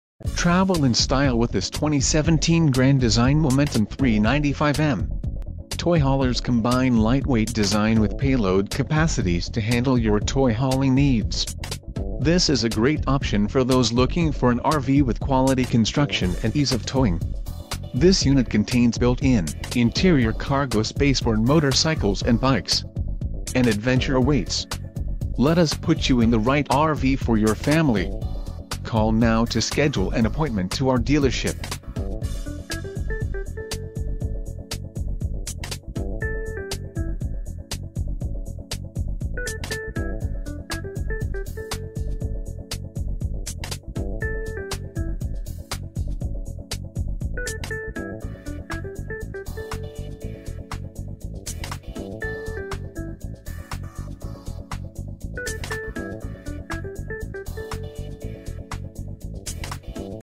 2017 Grand Design Momentum 395M - McClain's RV Superstore...
Grand Design has combined all of the best things about aluxury fifth wheel and combined them with the functionality of a toy hauler tocreate one incredible RV! The Momentum 395M is jam-packed with amazing featureslike slam-latch baggage doors, frameless tinted windows, exterior securitylights, exterior entertainmentcenter, and a power patio awning with integrated LED lighting.Inside, the Momentum 395M is full of luxurious amenitiessuch as recessed LED ceiling lighting, blue LED accent lighting, the Espresso dcor, andbeautiful raised panel hardwood cabinet doors with hidden hinges. Cooking lunchor dinner will be a breeze in this unique kitchen. The spacious solid surface countertopswith sink covers, 3-burner stainless steel stove top with oven below, and a 30stainless steel microwave will ensure you have all the tools you need!